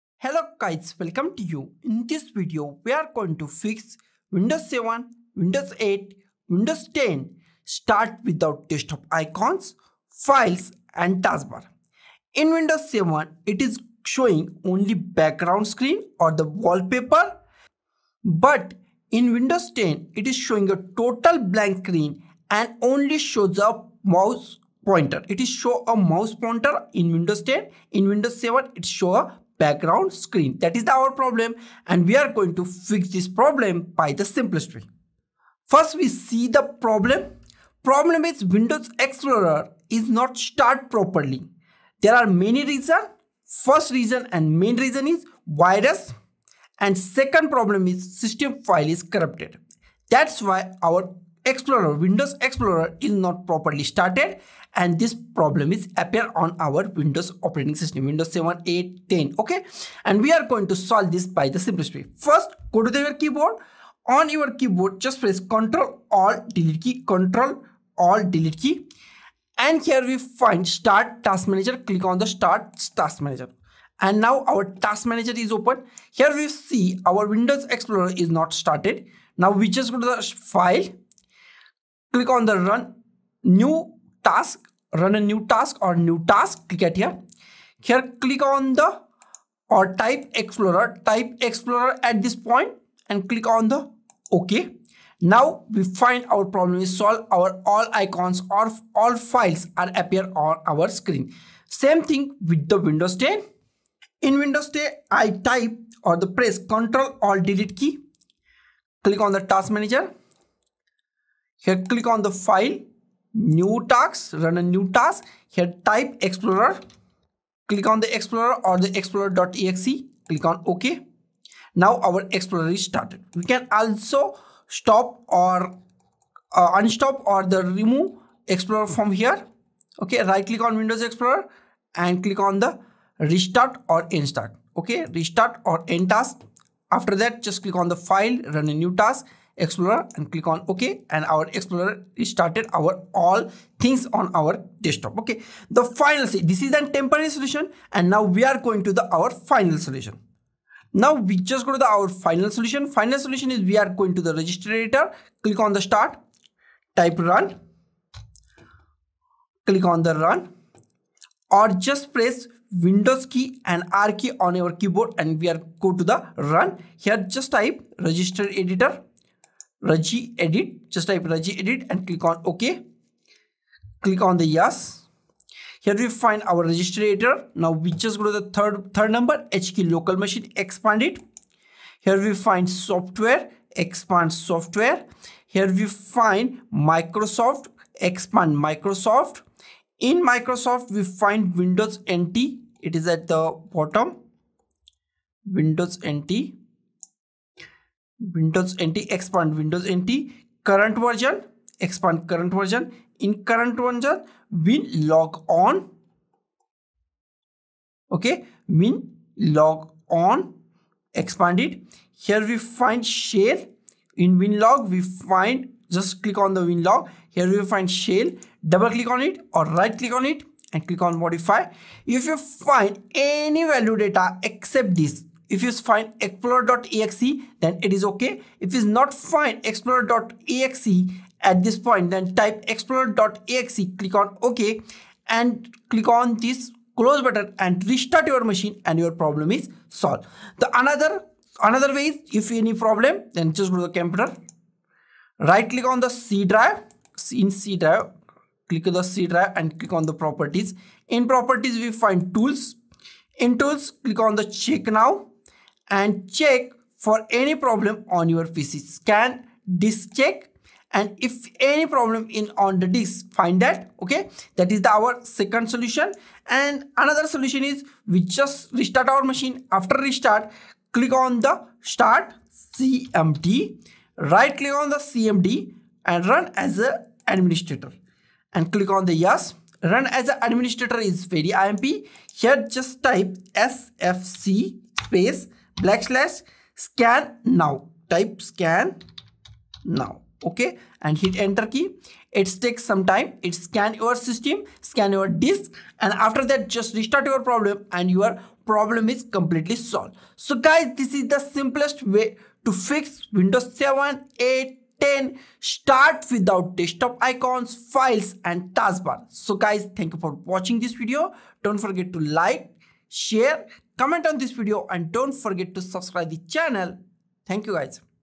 How to fix Windows 7, 8, 10 start without Desktop Icons Files & Taskbar - Windows Black Screen Error
Is your Windows 7, 8, or 10 PC starting up to a black screen with no desktop icons or taskbar? Don't worry; we've got you covered! In this comprehensive guide, we'll walk you through step-by-step instructions to troubleshoot and fix the Windows black screen error, so you can get your computer back to its normal functioning state.

Whether you're facing this issue on Windows 7, 8, or 10, our guide will help you diagnose the problem and provide effective solutions to get your desktop icons, files, and taskbar back where they belong.

Don't let the Windows black screen error disrupt your productivity any longer. Watch this video, follow the steps, and get your computer back in working order.